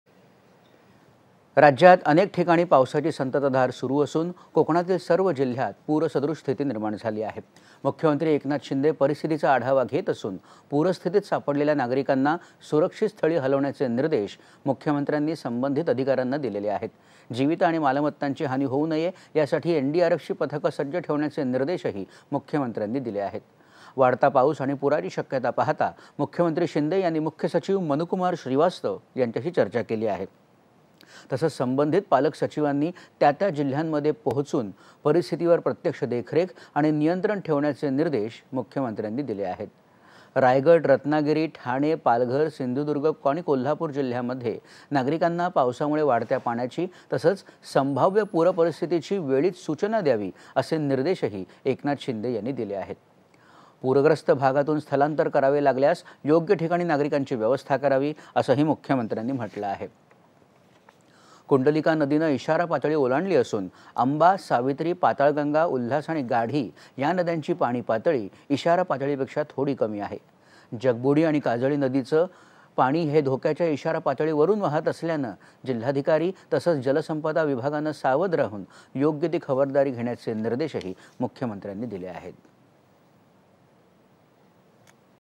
राज्यात अनेक ठिकाणी पावसाची संततधार सुरु
दि.०४.०७.२०२२ :
      राज्यात अनेक ठिकाणी पावसाची संततधार सुरु असून, कोकणातील सर्व जिल्ह्यात पूरसदृश स्थिती निर्माण झाली आहे. मुख्यमंत्री एकनाथ शिंदे परिस्थितीचा आढावा घेत असून, पूरपरिस्थितीत सापडलेल्या नागरिकांना सुरक्षित स्थळी हलवण्याचे निर्देश मुख्यमंत्र्यांनी संबंधित जिल्हाधिकाऱ्यांना दिले आहेत. जीवित आणि मालमत्तांची हानी होऊ नये, यासाठी एनडीआरएफची पथकं सज्ज ठेवण्याचे निर्देशही मुख्यमंत्र्यांनी दिले आहेत.
     वाढता पाऊस आणि पूराची शक्यता पाहता, मुख्यमंत्री शिंदे यांनी मुख्य सचिव मनुकुमार श्रीवास्तव यांच्याशी चर्चा केली आहे. तसंच संबंधित पालक सचिवांनी त्या त्या जिल्ह्यांमध्ये पोहोचून परिस्थितीवर प्रत्यक्ष देखरेख आणि नियंत्रण ठेवण्याचे निर्देश मुख्यमंत्र्यांनी दिले आहेत.
     रायगड, रत्नागिरी, ठाणे, पालघर, सिंधुदुर्ग आणि कोल्हापूर जिल्ह्यांमध्ये नागरिकांना पावसामुळे वाढत्या पाण्याची तसंच संभाव्य पूरपरिस्थितीची वेळीच सूचना द्यावी, असे निर्देशही एकनाथ शिंदे यांनी दिले आहेत.
     पूरग्रस्त भागातून स्थलांतर करावे लागल्यास योग्य ठिकाणी नागरिकांची व्यवस्था करावी, असंह मुख्यमंत्र्यांनी म्हटलं आहे.
     कुंडलिका नदीनं इशारा पातळी ओलांडली असून अंबा, सावित्री , पातळगंगा, उल्हास आणि गाढी या नद्यांची पाणीपातळी इशारा पातळीपेक्षा थोडी कमी आहे.
     जगबुडी आणि काजळी नदीचं पाणी धोक्याच्या इशारा पातळीवरुन वाहत असल्यानं जिल्हाधिकारी तसंच जलसंपदा विभागानं सावध राहून योग्य ती खबरदारीचे घेण्याचे निर्देशही मुख्यमंत्र्यांनी दिले आहेत.